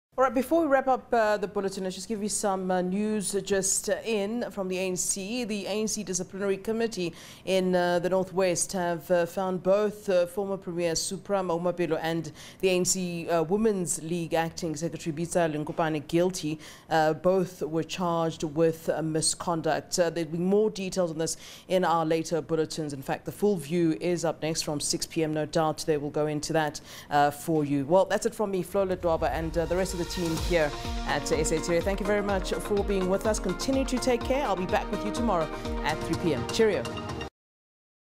BREAKING NEWS | Supra Mahumapelo suspended from ANC for 5 years
The ANC Disciplinary Committee in the North West, have found both former premier Supra Mahumapelo and ANC Women's League Acting Secretary, Bitsa Lenkopane guilty. Both were charged with misconduct.